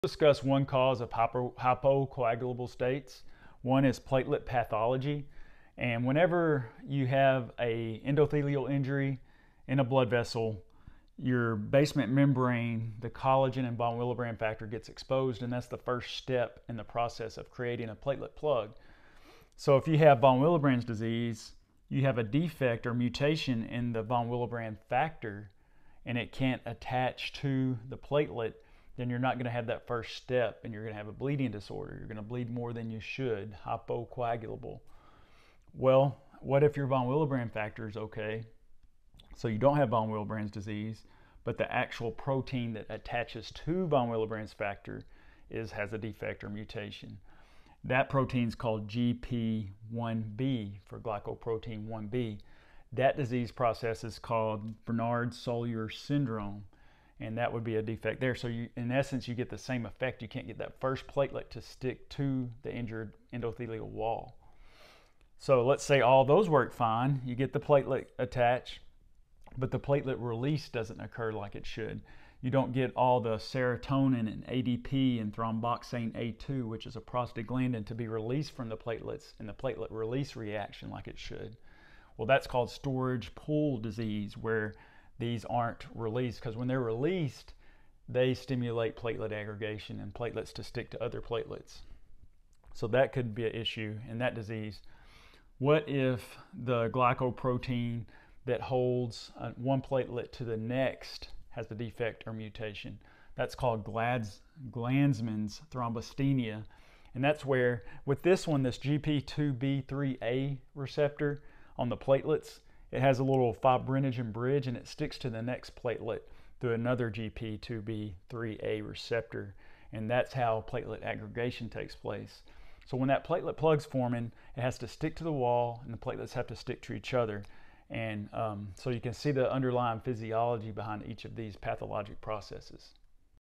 Bernard-Soulier Syndrome vs Glanzmann
Discusses platelet pathophysiology involving Von Willebrand factor, GPIb receptors, GPIIa/IIIb receptors, and release of ADP, serotonin, and thromboxane.